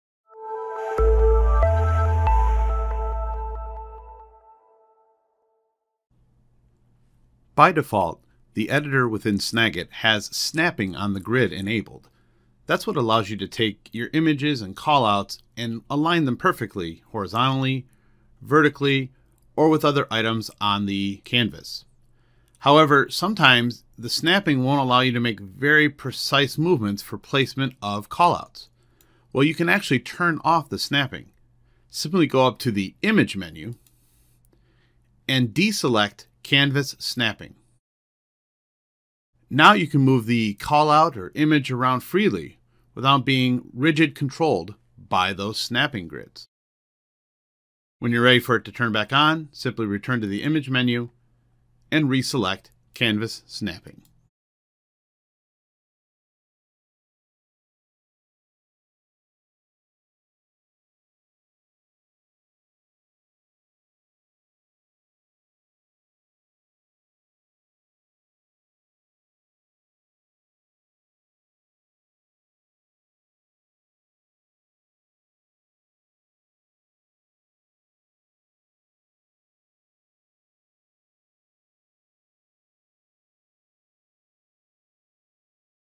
Turn Canvas Snapping On or Off in Snagit
In the TechSmith Snagit Editor, Canvas Snapping helps to automatically align objects on your canvas. At times, snapping can interfere with finely controlling placement of an object. To turn Canvas Snapping off, go to the Image menu. Then uncheck Canvas Snapping. This feature can be re-enabled by going back to the Image menu and select Canvas Snapping.